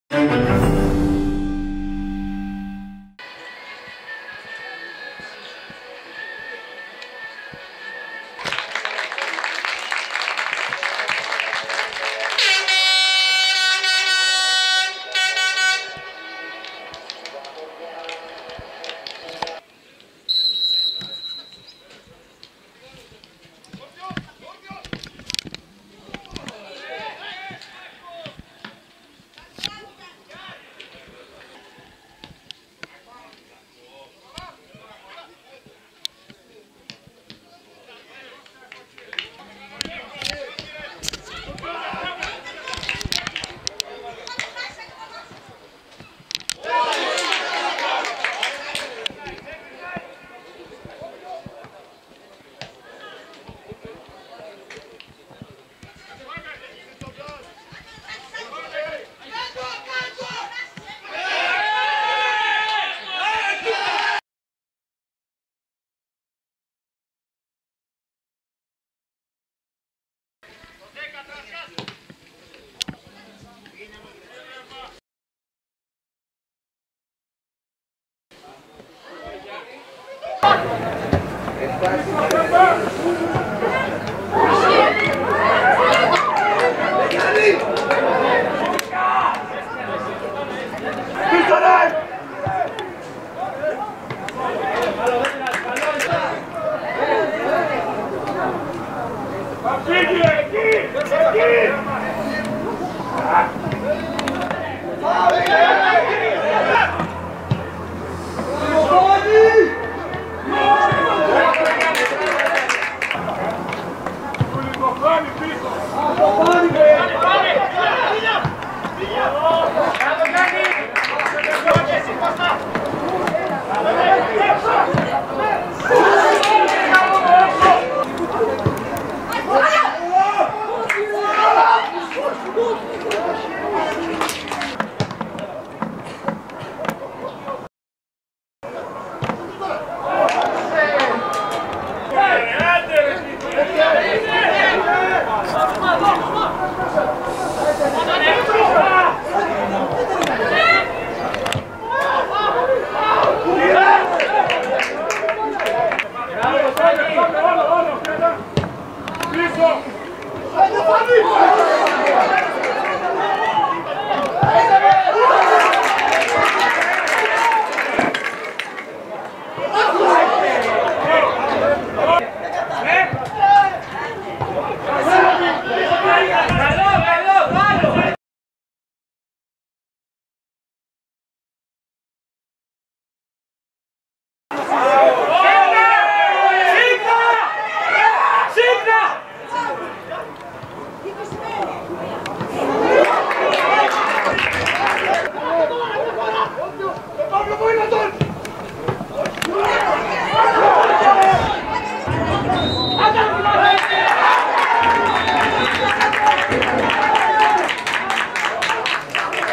Κύπελλο - Α΄ φάση: Τα highlights του αγώνα Μέγας Αλέξανδρος Καλλιθέας - Πεντάβρυσος (0-0)
Οι καλύτερες στιγμές και οι αμφισβητούμενες φάσεις της αναμέτρησης στο ΔΑΚ Καστοριάς, μεταξύ του Μέγα Αλέξανδρου Καλλιθέας και της Πενταβρύσου (0-0), στα πλαίσια της 1ης αγωνιστικής της α΄ φάσης του κυπέλλου της ΕΠΣ Καστοριάς.